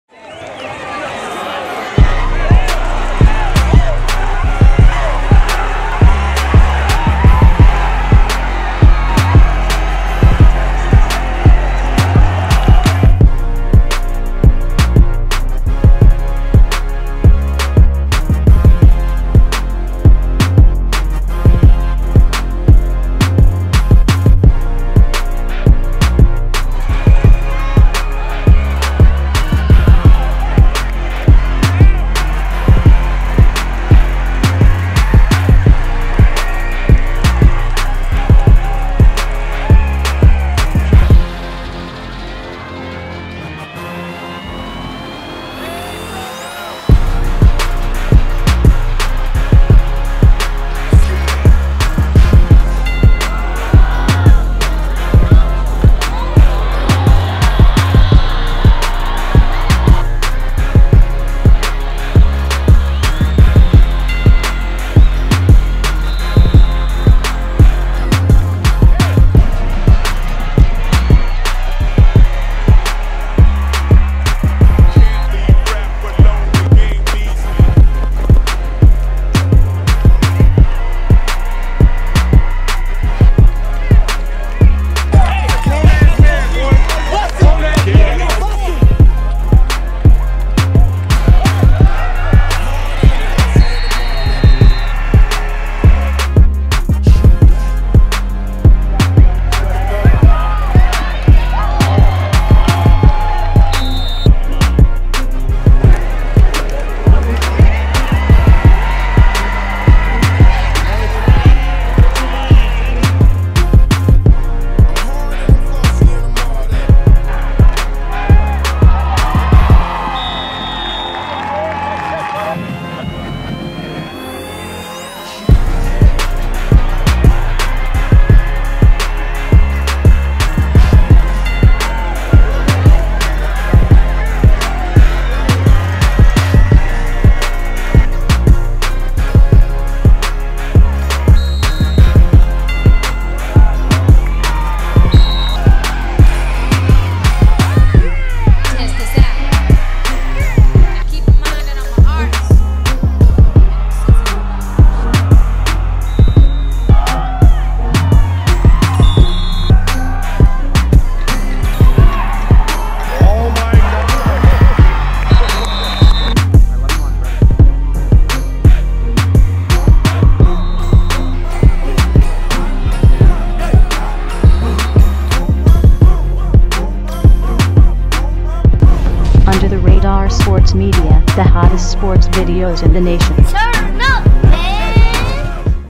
One of The BEST RBs in the Country | 🔥🔥 George Holani '19 | St John Bosco (CA) Senior Highlight Mix
🔥🔥 One of The BEST RBs in the Country | George Holani '19 | St John Bosco (CA) Senior Year Highlight Mix